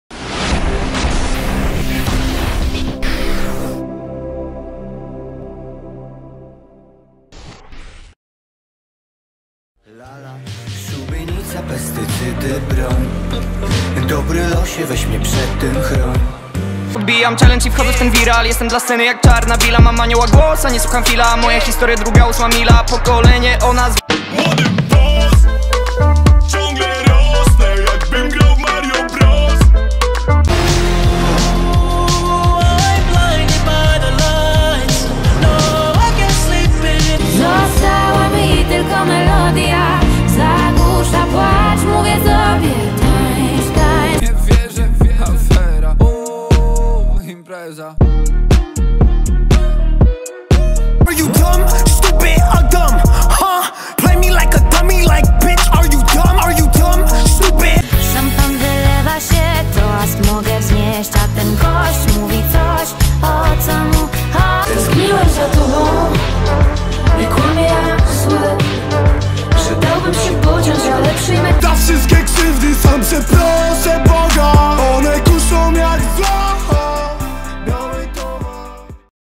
Polish Singles Chart | Top 10 | 2020.05.24
Week: May 15, 2020 - May 21 2020

Early release of 100 biggest music hits right now in Poland.
The chart is based on streaming and airplay data